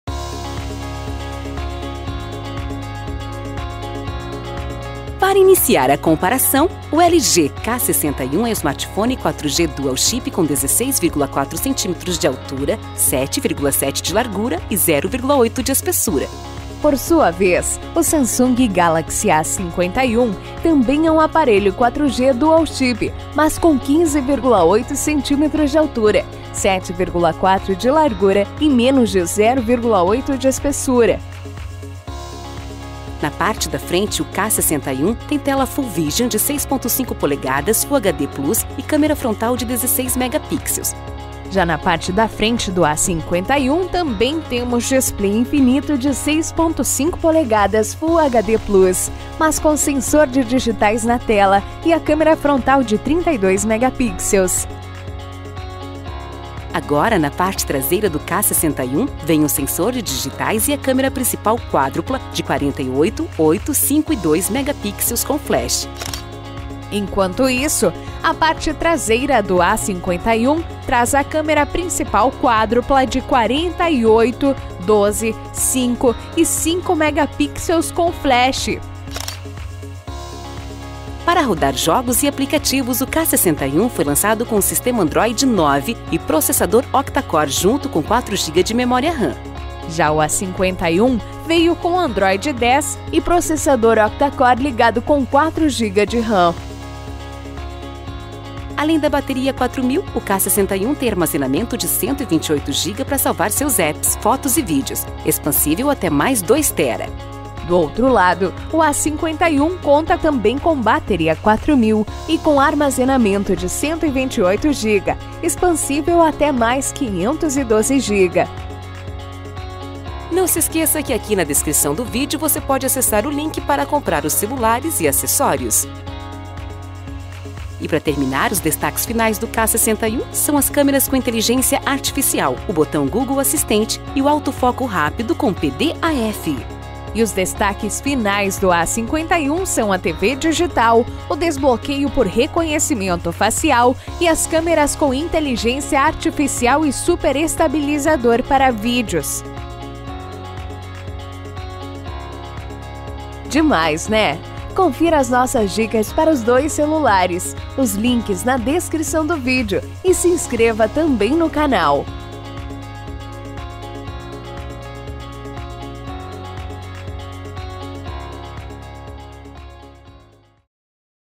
LG K61 vs A51 (Comparativo)
Confira a ficha técnica do LG K61 e do Samsung Galaxy A51, uma comparação de tamanho exclusiva (3D), e muito mais.

Links para comprar:

(Galaxy A51)